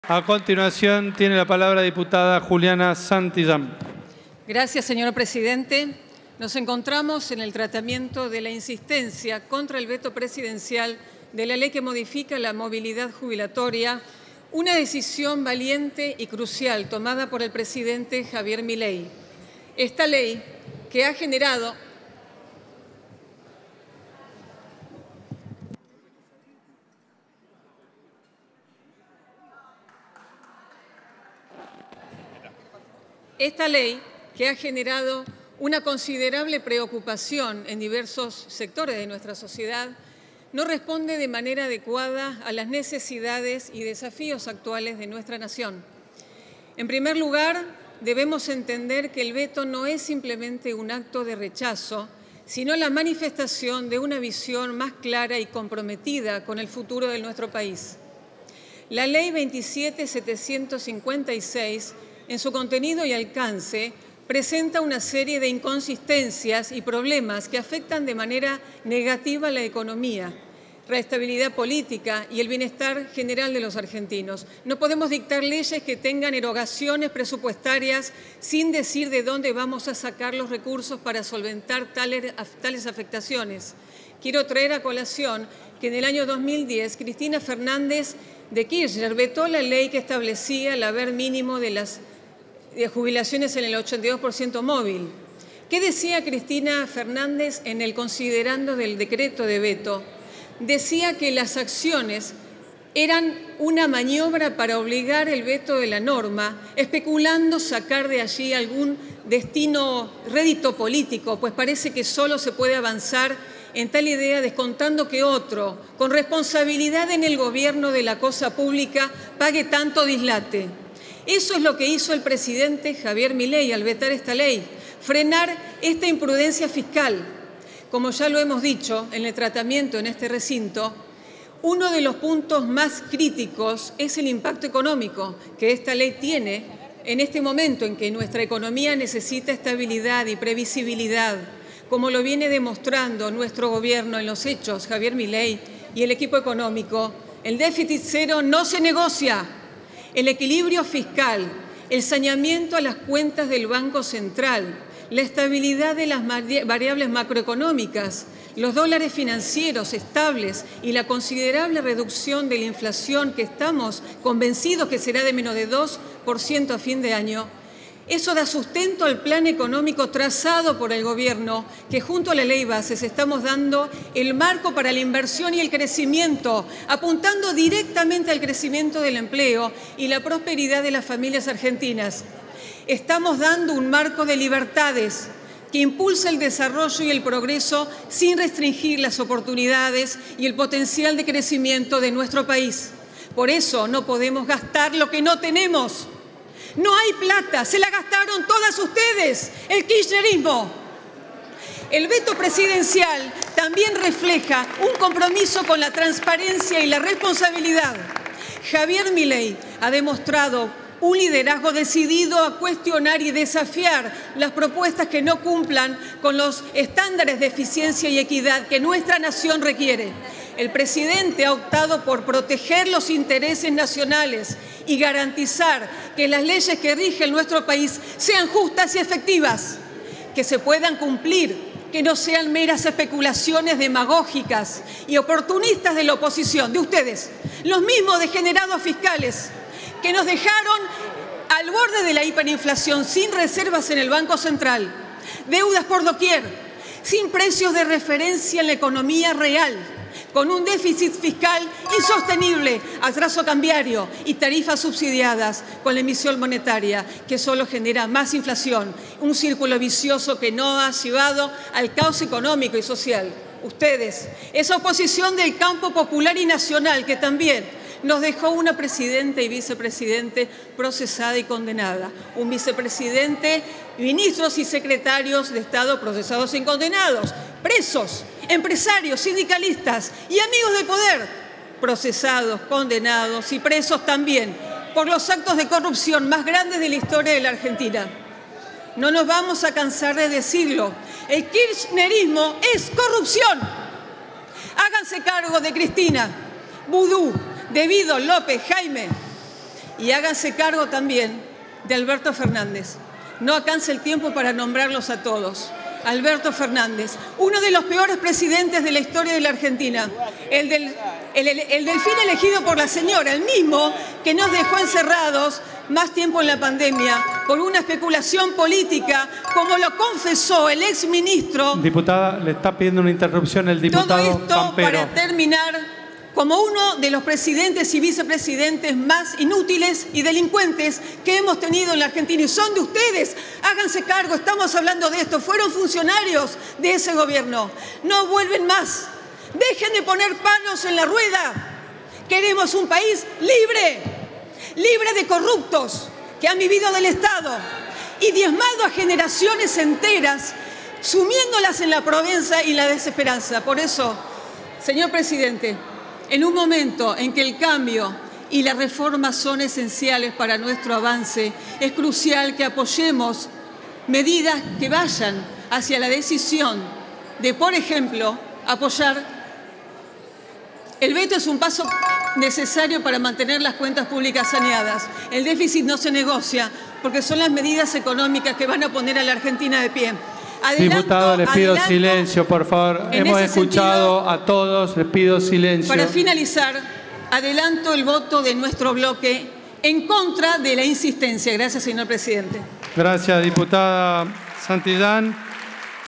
Diputada Santillán Juárez Brahim, Juliana - LLA - Sesión 11-09-2024
A fin de considerar el siguiente:
◽ - Expediente 𝟭𝟱-𝗣𝗘-𝟮𝟬𝟮𝟰 Mensaje 𝟰𝟱/𝟮𝟰 comunicando el dictado del decreto 𝟳𝟴𝟮 por el cual se observa totalmente y se devuelve el proyecto de ley registrado bajo el número 𝟮𝟳.𝟳𝟱𝟲.

República Argentina